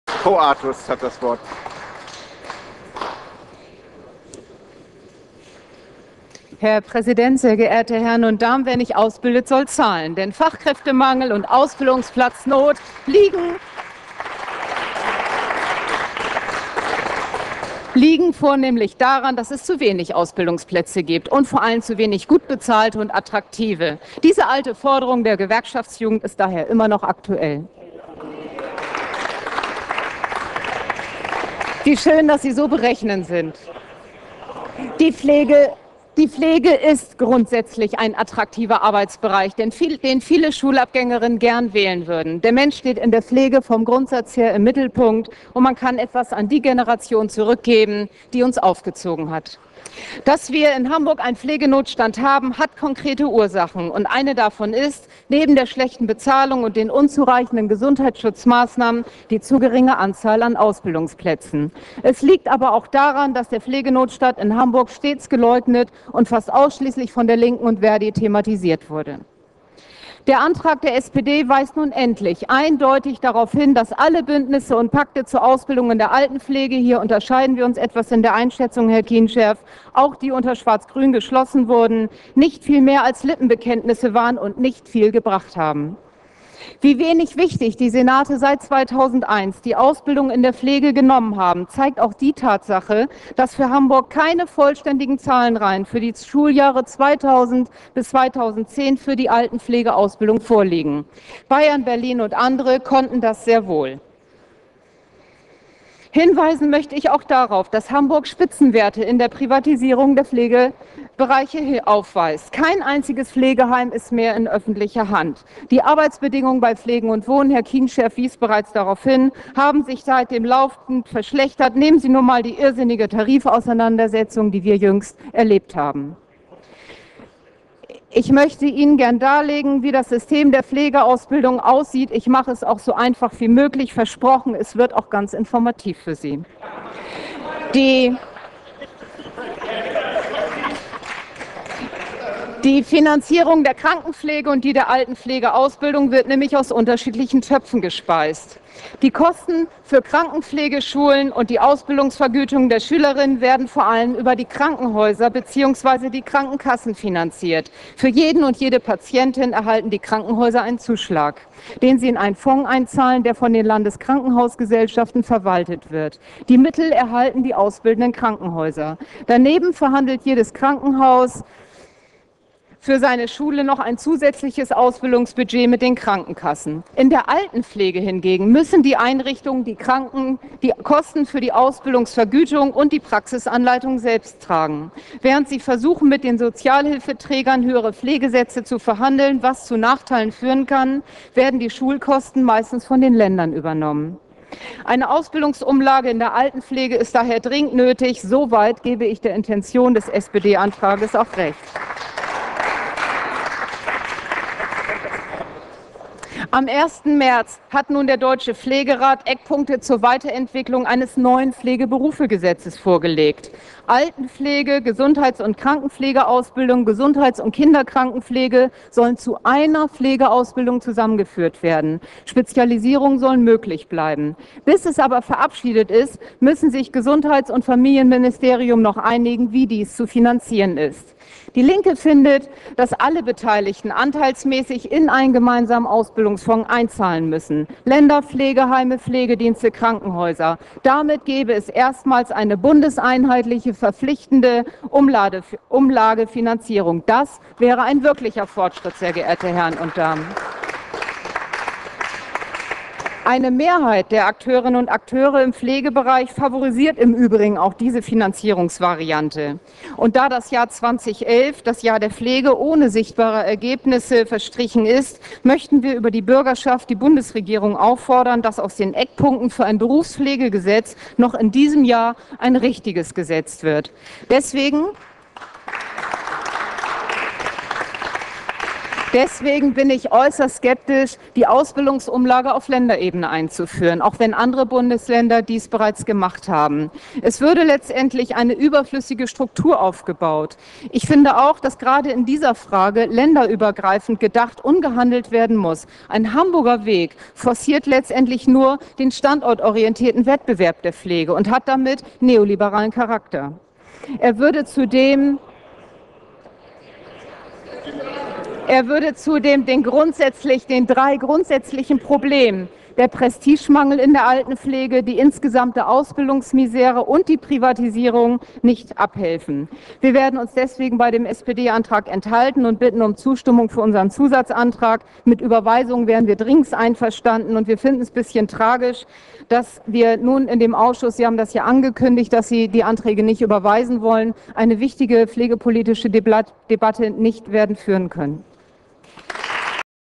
Ausbildungsumlage für die Altenpflege
Rede in der Hamburgischen Bürgerschaft am 14. Juni 2012 zur Ausbildungsumlage in der Altenpflege. Wer nicht ausbildet, soll zahlen! DIE LINKE fordert eine Umlage auf Bundesebene. Ein Ländergesetz kann zur Folge haben, dass der Wettbewerb angeheizt wird, der durch die Privatisierung in der Pflege existziert. Dann hätte eine Ulage auf Landesebene neoliberalten Charakter.